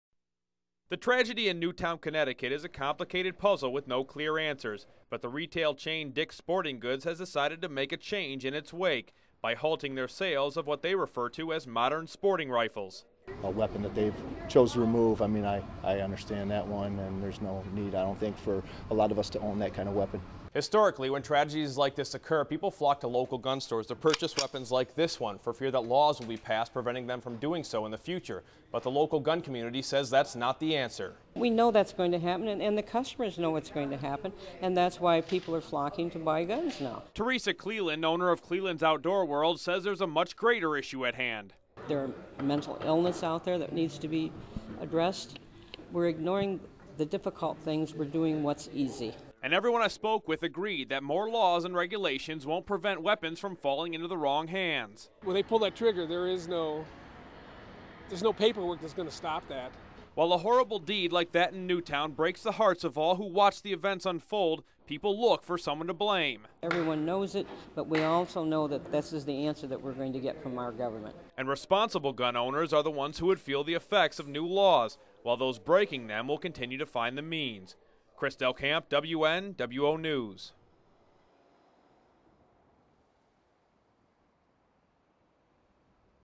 Dick's Sporting Goods halts gun sales
Dick's Sporting Goods halts gun sales in light of the Newtown, Connecticut massacre.